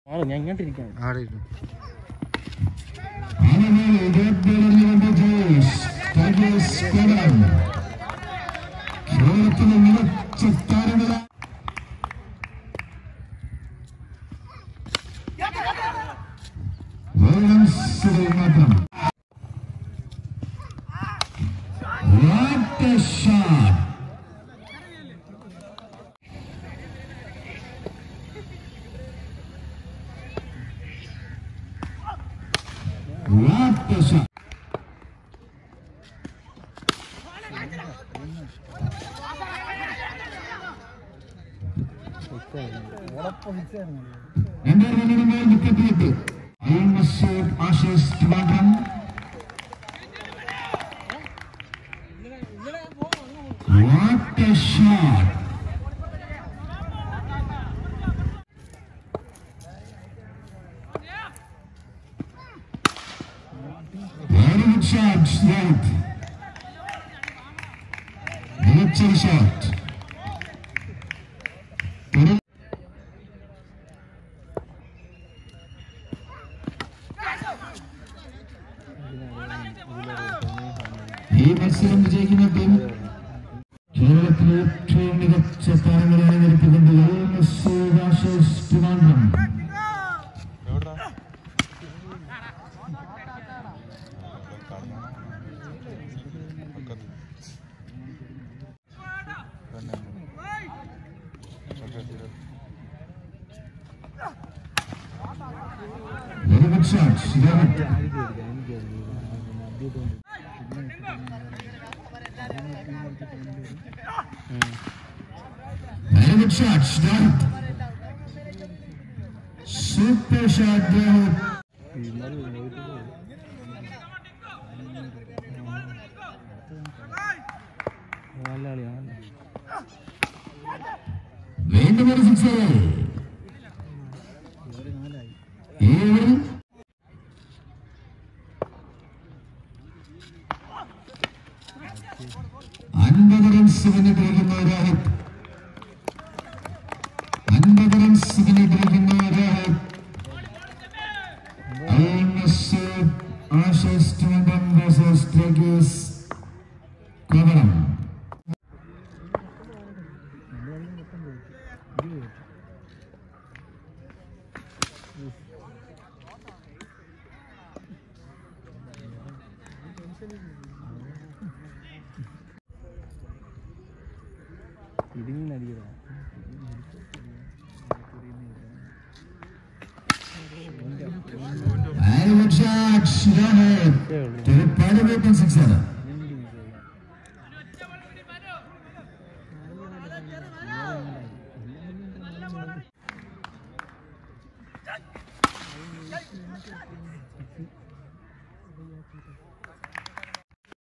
ROHITH ATTINGAL BATTING KMCC VAKKOM CRICKET TOURNAMENT ASHES VS STRICKERS KOVALAM 22/12/20
TENNIS BALL CRICKET